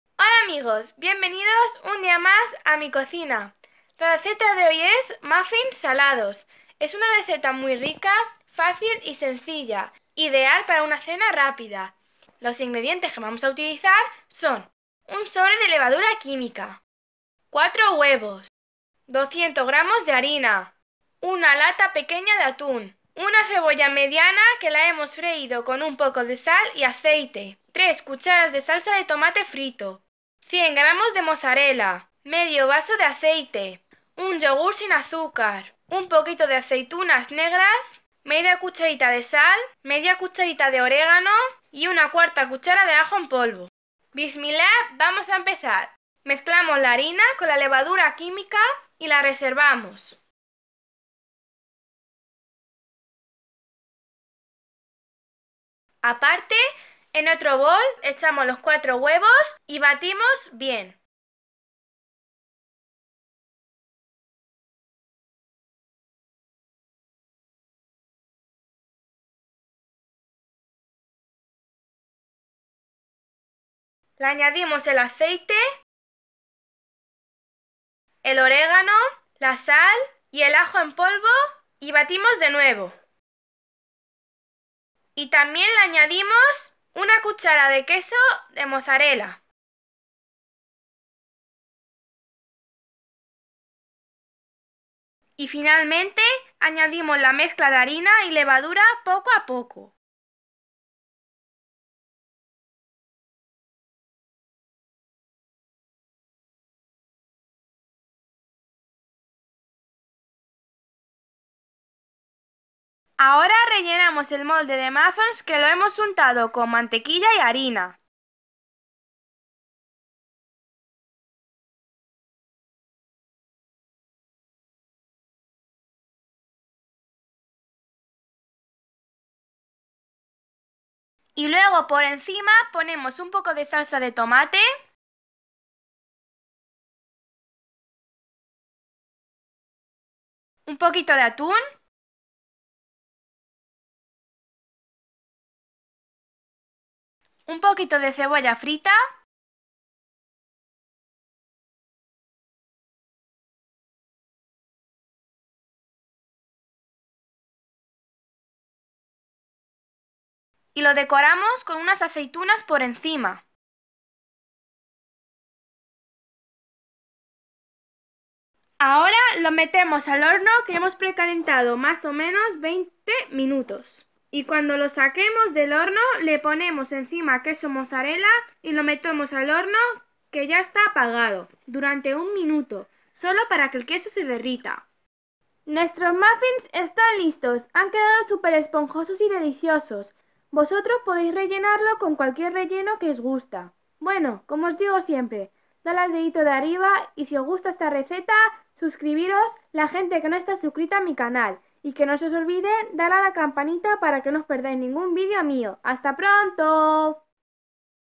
Muffins salados ميني كيك مالح /comida de marruecos
LOS INGREDIENTES

- 200 g de harina
- 4 huevos
- un sobre de levadura química
- un yogur
- 1/2 vaso de aceite
- una lata de atún
- 3 c g de tomate frito
- una cebolla
- 100 g de queso
- 1/2 c p de sal
- 1/2 c p de orégano
- 1/4 c p de ajo en polvo
- Aceitunas negras